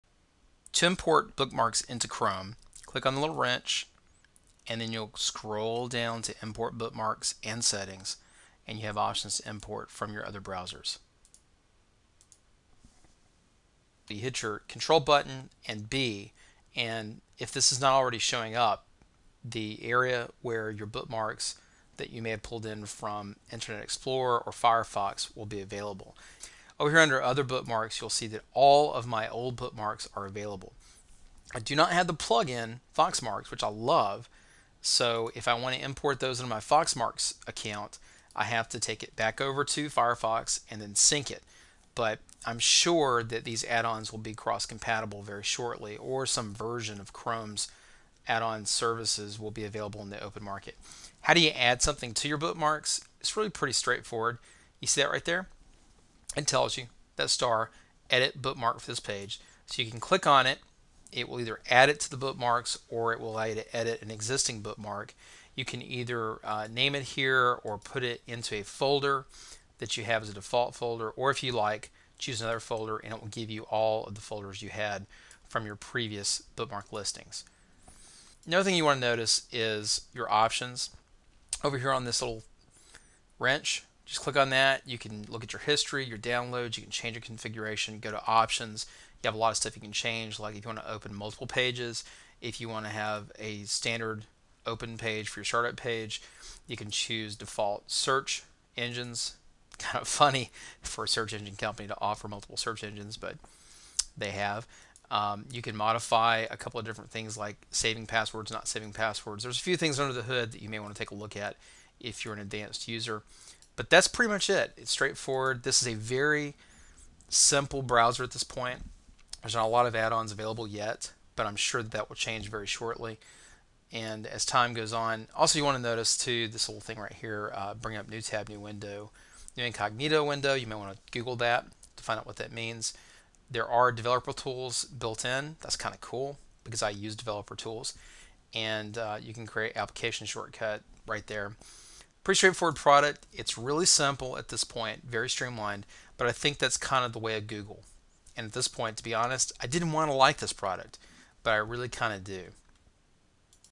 Chrome Bookmarks
Importing, editing, adding, and using bookmarks in Google Chrome.